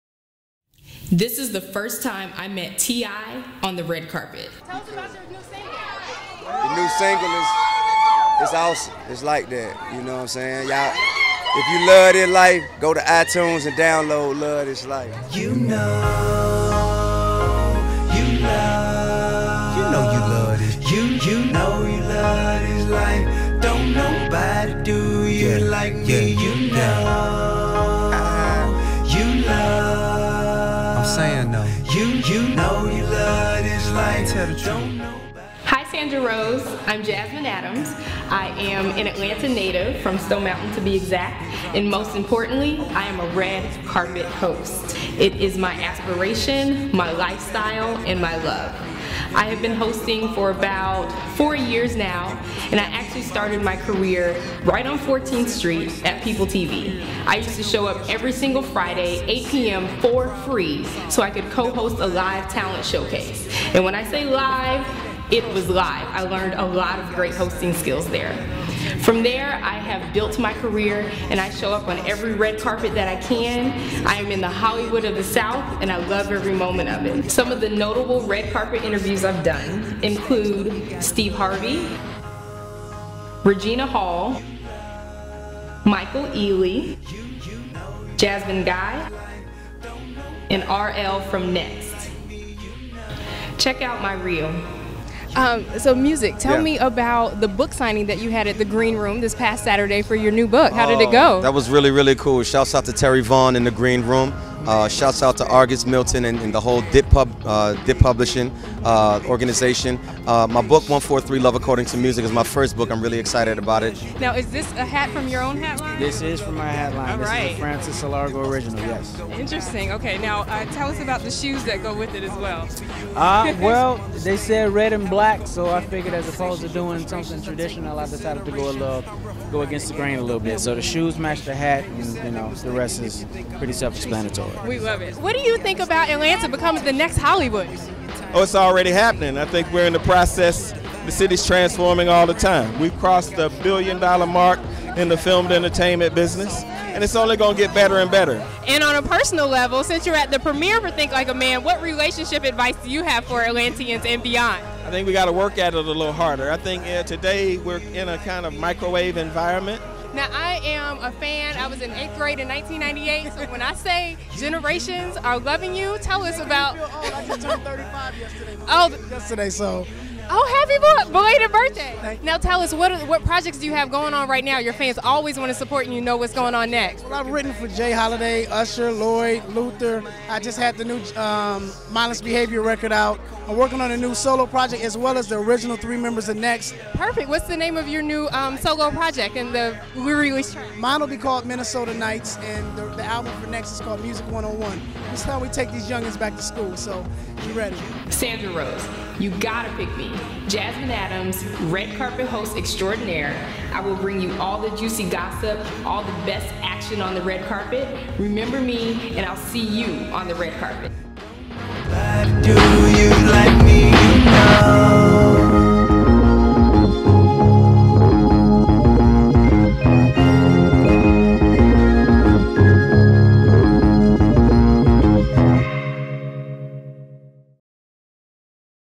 Jasmine Adams - Red Carpet Host Extraordinaire
Jasmine Adams is a red carpet host extraordinaire. It is her aspiration, her lifestyle, and her love. She continues to build her career by showing up on every red carpet in the "Hollywood of the South," Atlanta, GA.

Videographer: Khalil Hasan - Team Ten Media
Editor: Richard Kearns - Team Ten Media